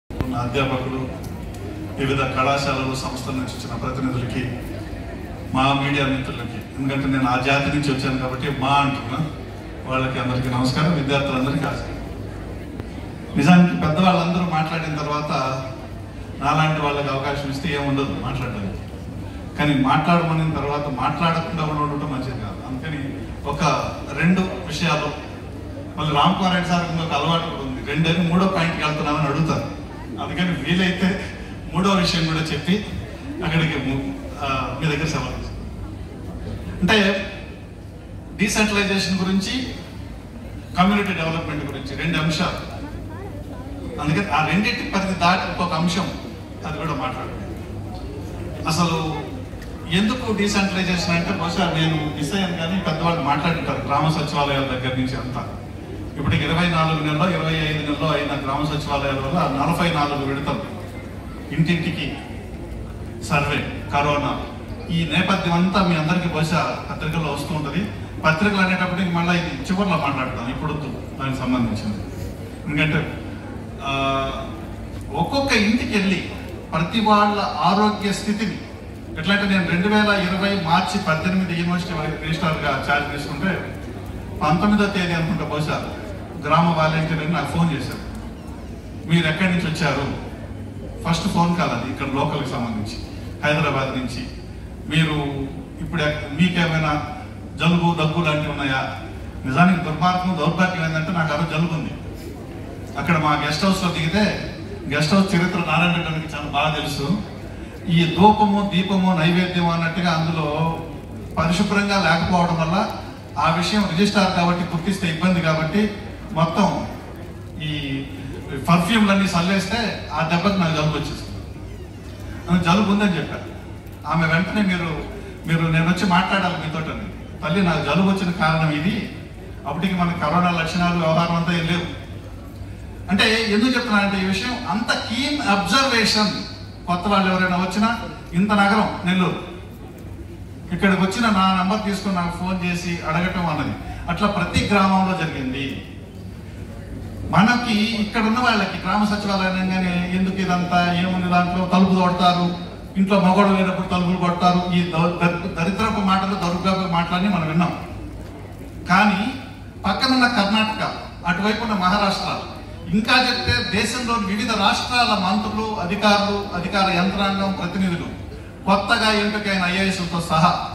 SPEECH BY CHIEF GUEST OF THE OCCASION REGISTRAR PROF.LV KRISHNA REDDY IN THE OCCASION OF CDP
SPEECH BY CHIEF GUEST OF THE OCCASION REGISTRAR PROF.LV KRISHNA REDDY IN THE  OCCASION OF A FOCUS GROUP DISCUSSION OF DECENTRALIZATION EASE OF GOVERNANCE